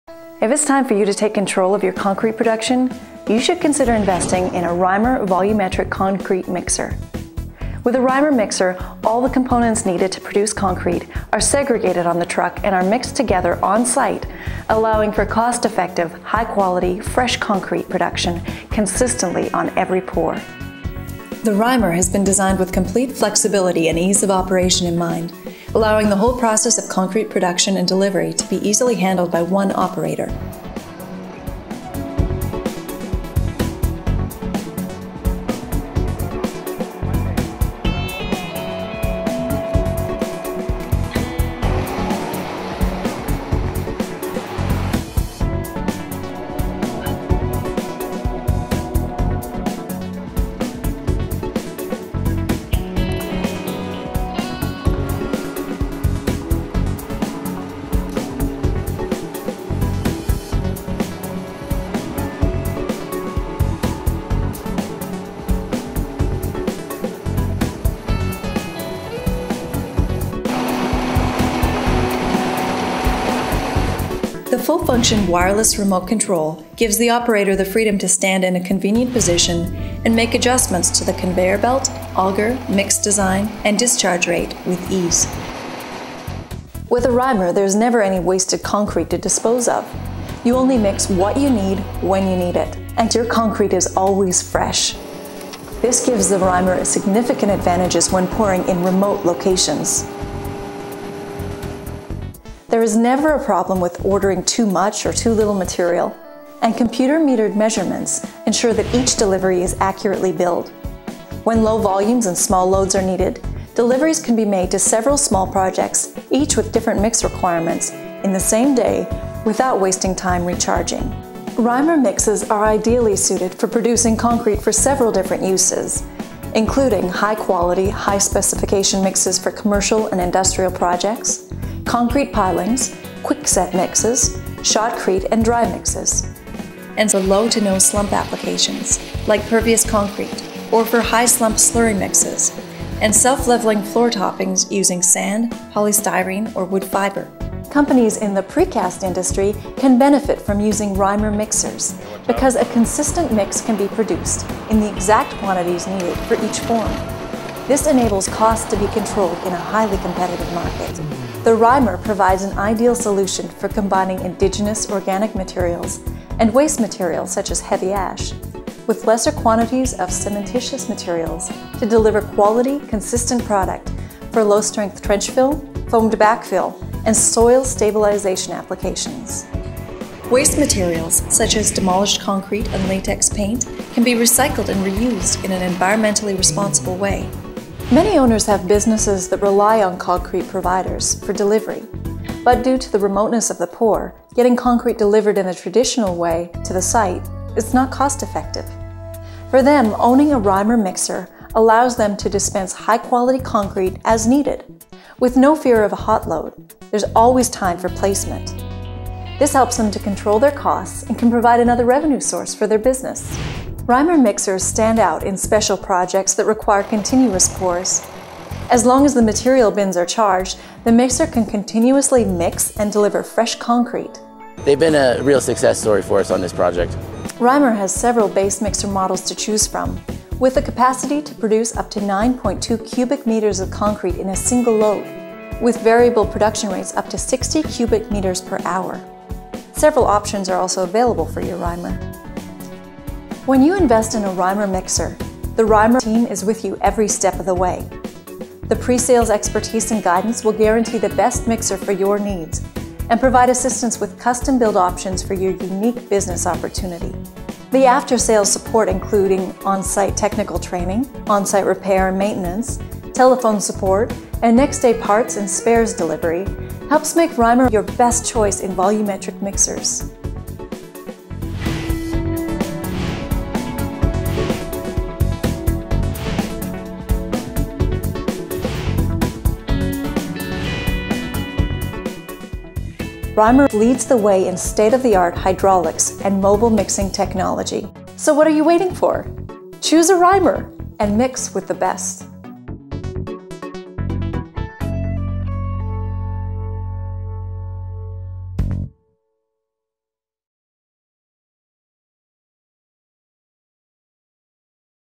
ProAll Reimer Mixers: An Introduction
The Reimer Mixer, ProAll’s flagship product, creates specialized solutions for quality, cost-effective on-site mixing. All of the components – stone, sand, cement and water – are loaded into separate compartments on one truck, trailer, or static-mounted unit. The components are metered and mixed into fresh concrete in a high-production mixing auger for a consistent, fresh mix anywhere you want it, at any time. Continuous mixing technology, continuous possibilities.